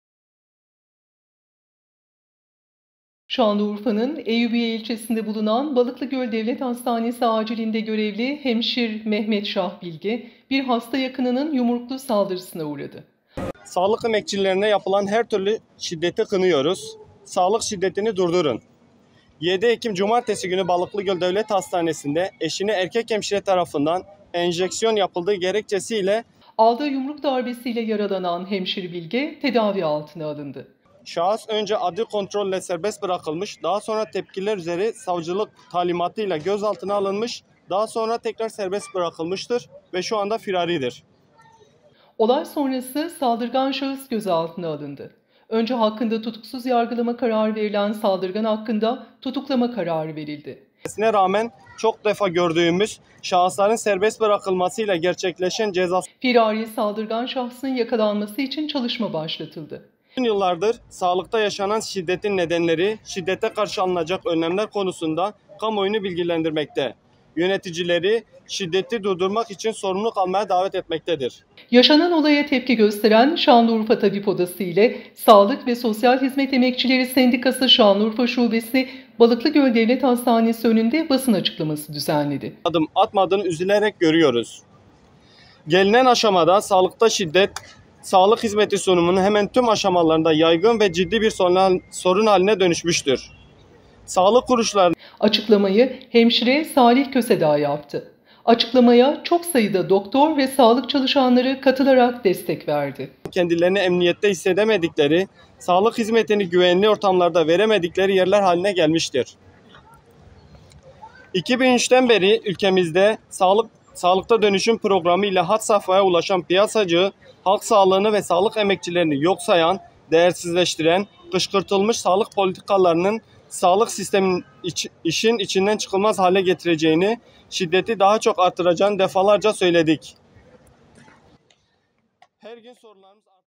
Sağlık Çalışanlarından Şiddete Tepki
Şanlıurfa Tabip Odası ile Sağlık ve Sosyal Hizmet Emekçileri Sendikası Şanlıurfa Şubesi, Balıklıgöl Devlet Hastanesi’nde darp edilen hemşir ile ilgili basın açıklaması düzenledi.